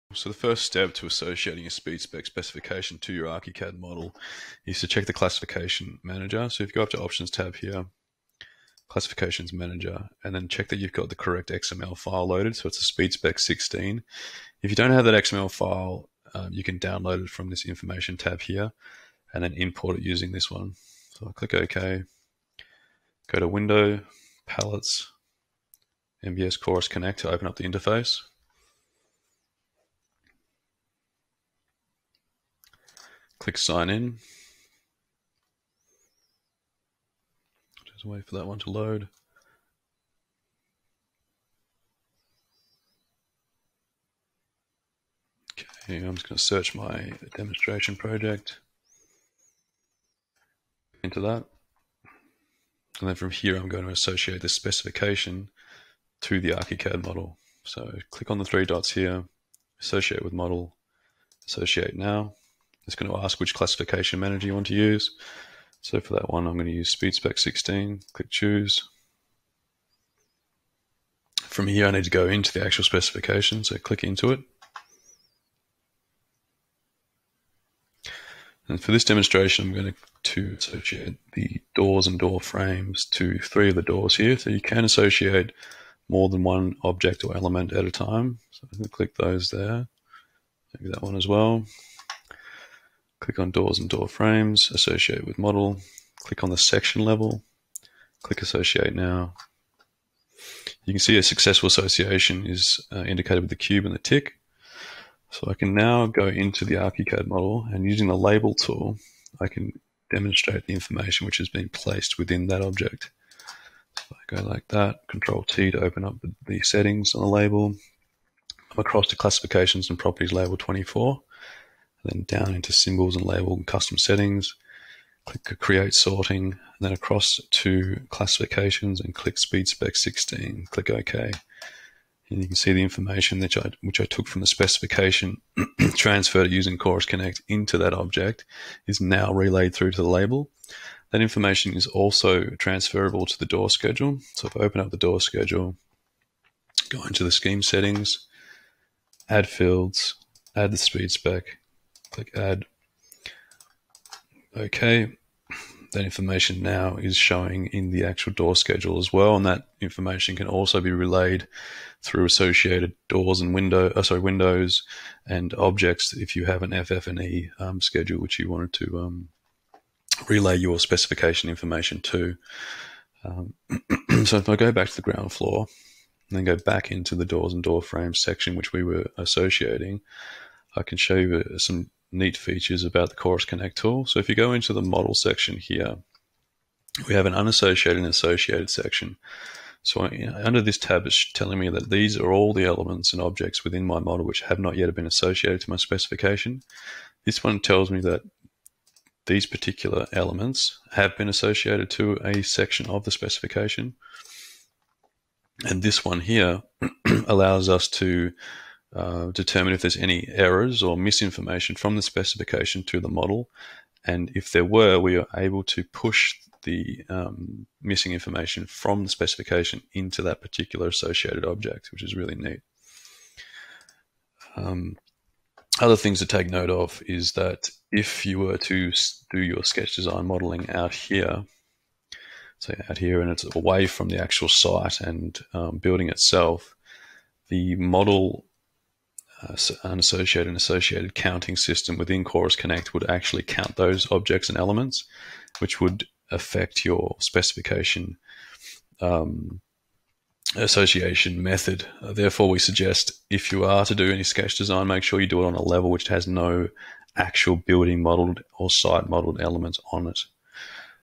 SpeedSpec in NBS Chorus - ArchiCAD associations and model checking
NBS Chorus enables you to develop your specification directly within Graphisoft ArchiCAD.

Learn how you can keep your information coordinated to improve accuracy and reduce risk.